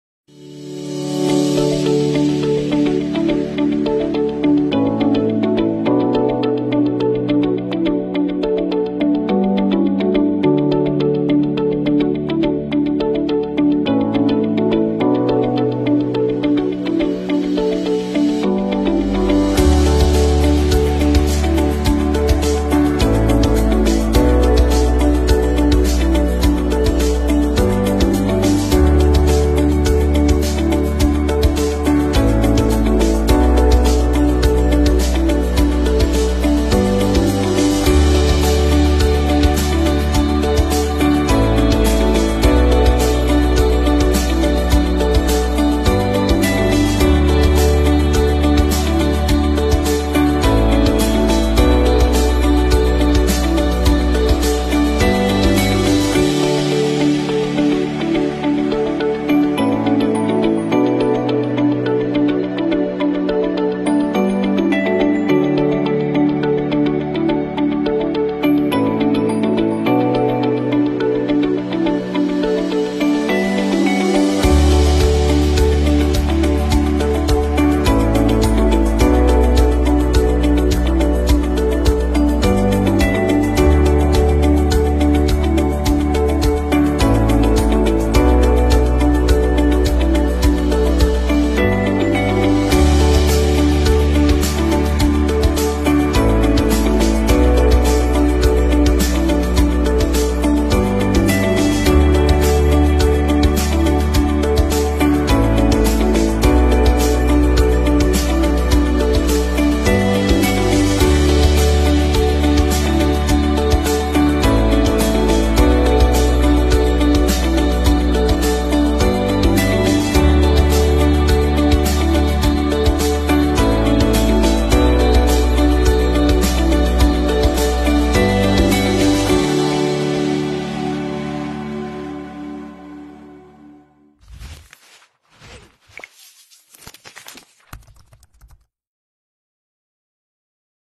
Take An Immersive Experience Into A Day at The Sublistar Factory!
The factory is fully equipped with production parts, machine assembly, crating, and transportation, all of which are specialized, refined, and visualized.